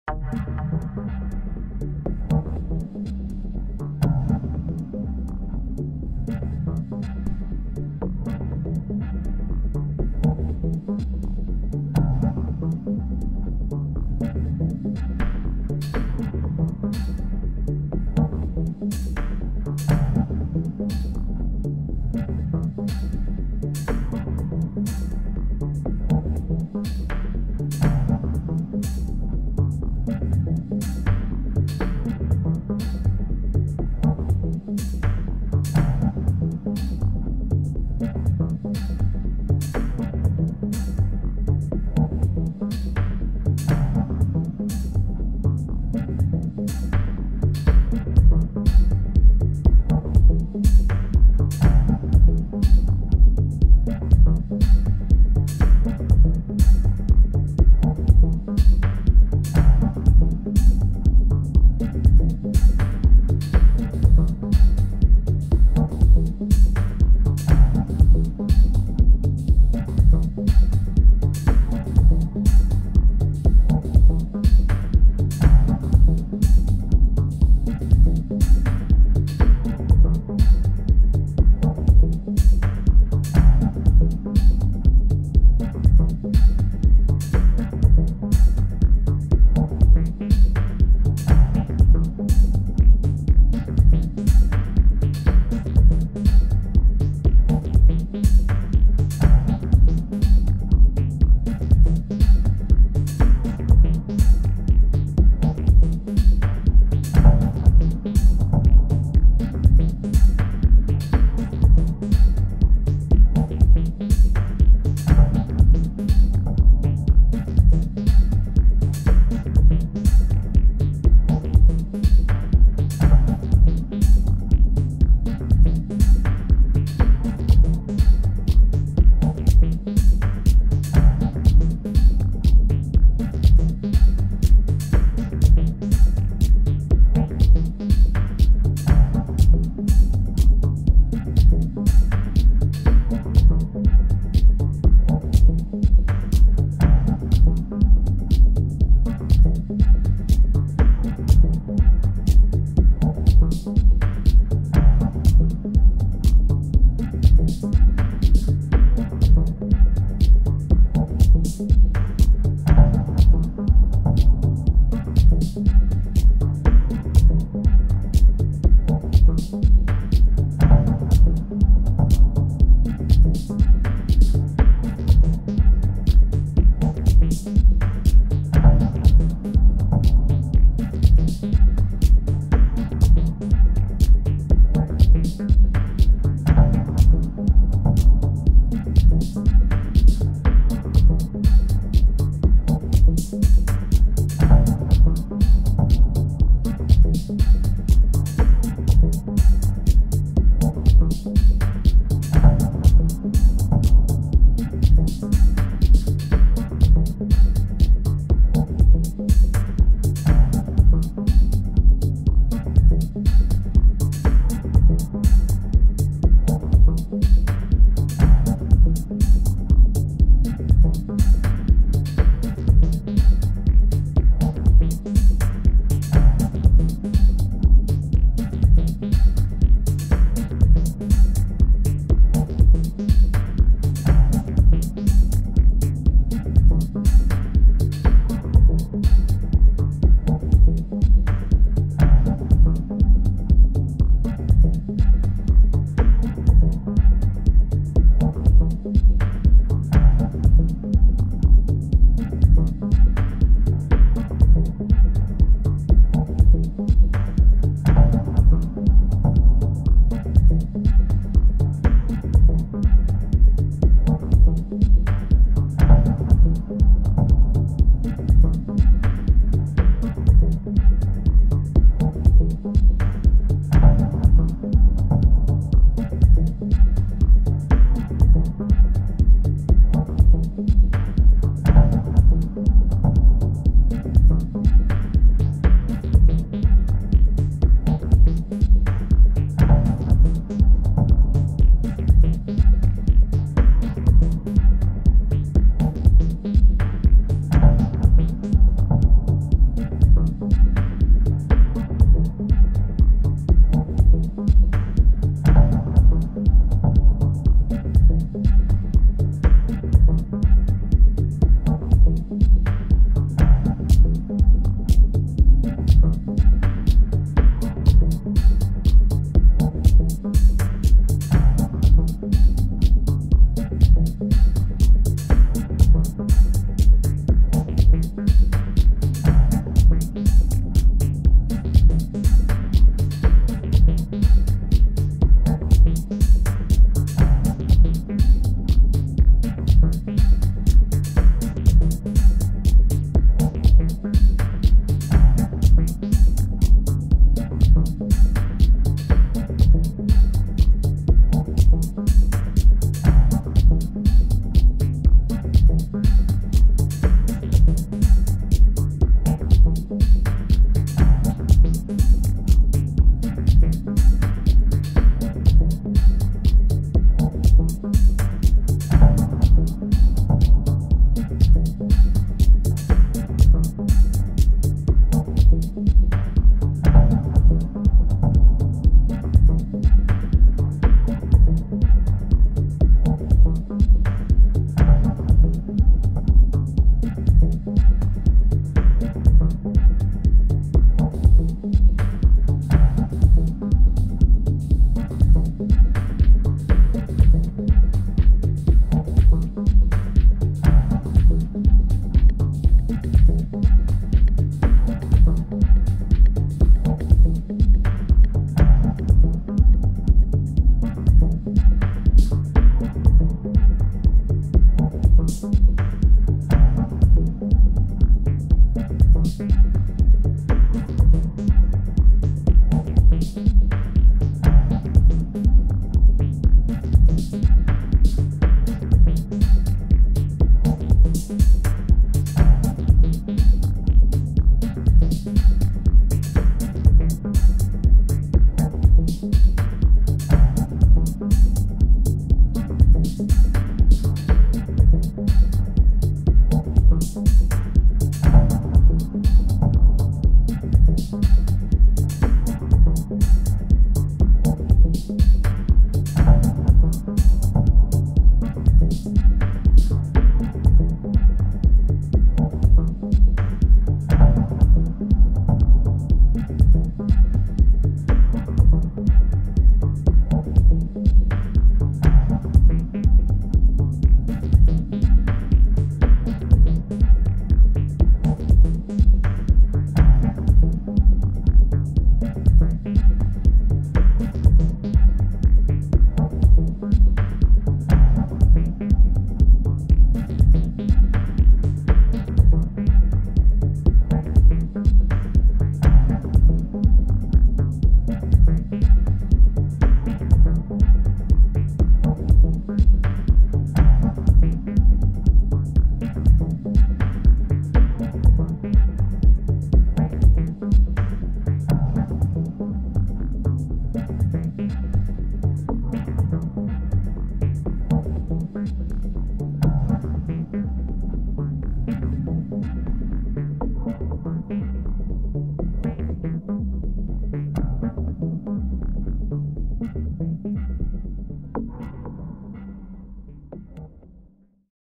automated navigation - live acid dub techno - sp-16 - tr6s - tr08 - tr06 - jd08 - se02 - tb03 - as-1
live dub techno + acid - sp-16 - tr6s - tr-08 - tr-06 - jd-08 - se-02 - tb-03 - as-1 - re-202 space echo + another similar chain of mine, dual tb-03/se-02 by cv/gate and the toraiz as-1 getting triggered on weird ways by the tr-06, jd-08 still using midi clock..., don't even know way...